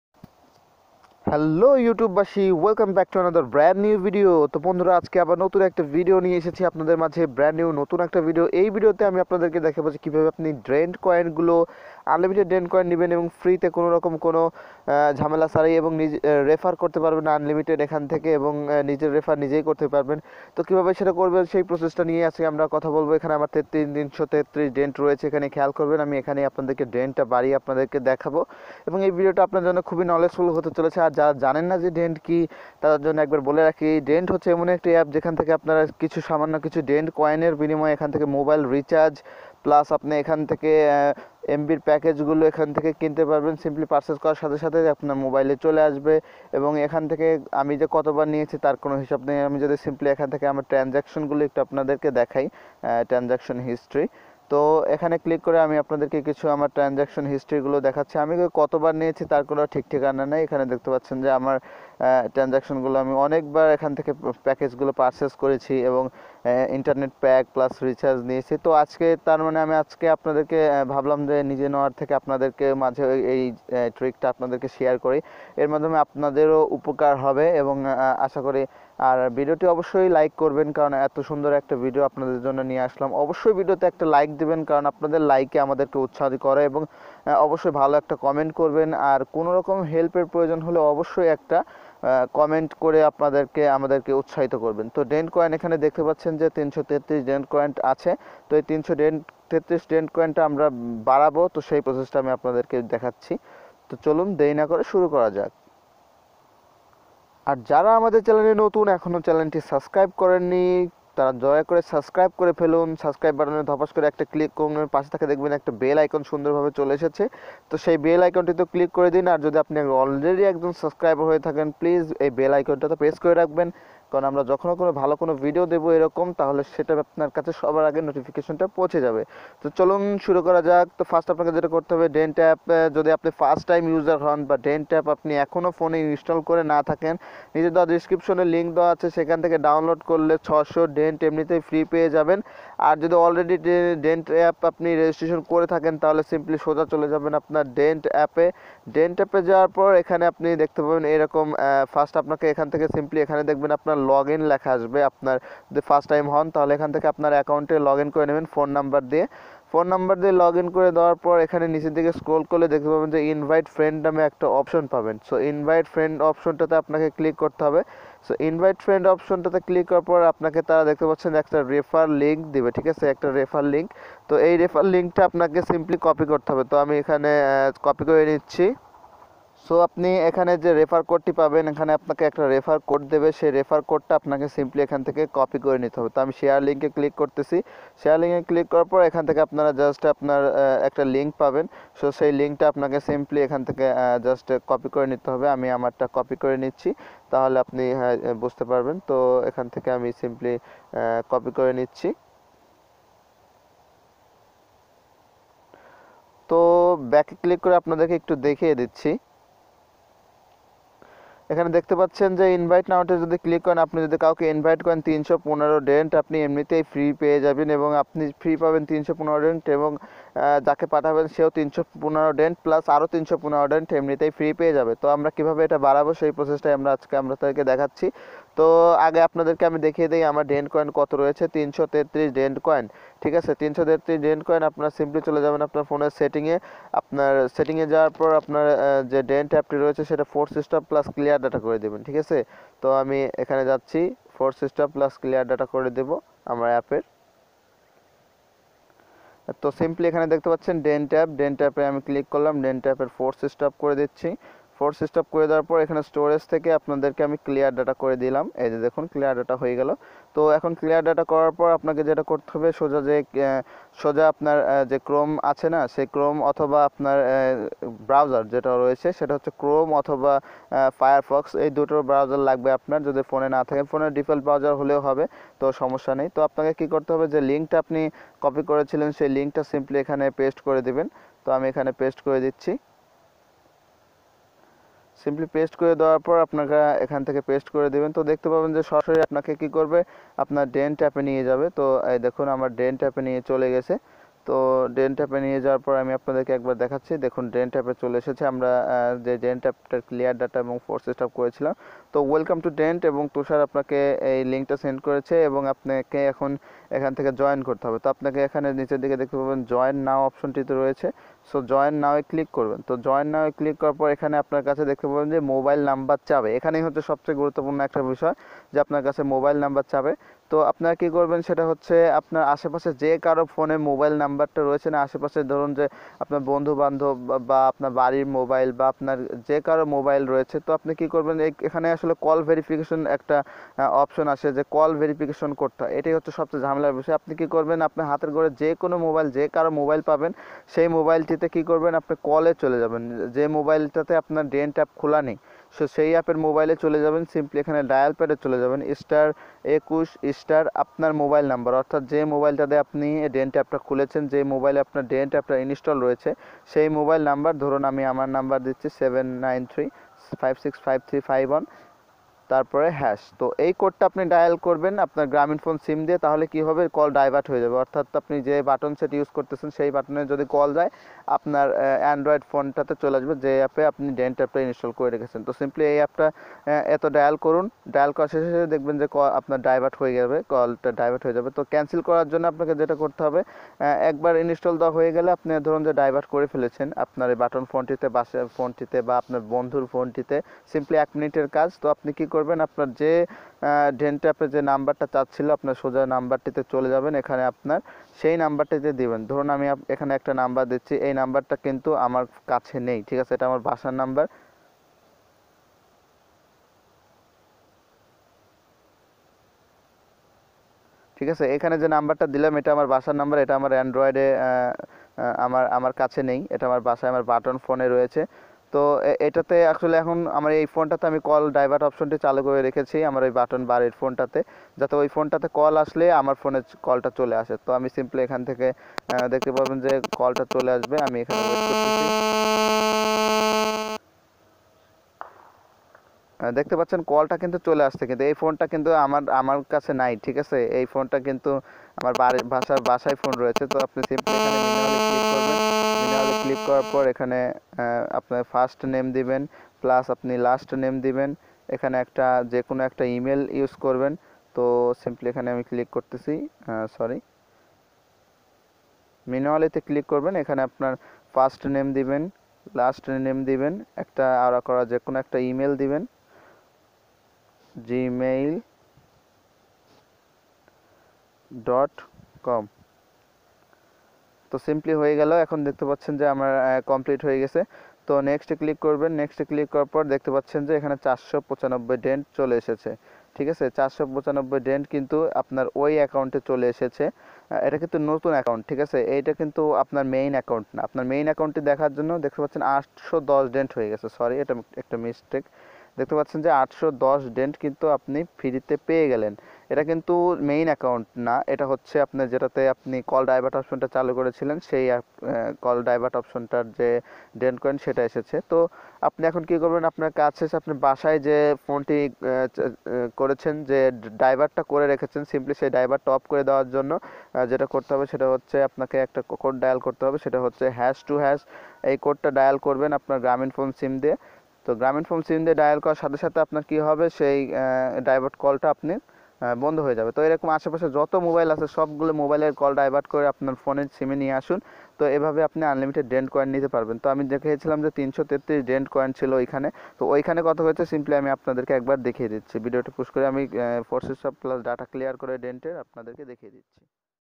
Dent app back get unlimited free dent with 100 taka mobile recharge
Best of bangla youtube school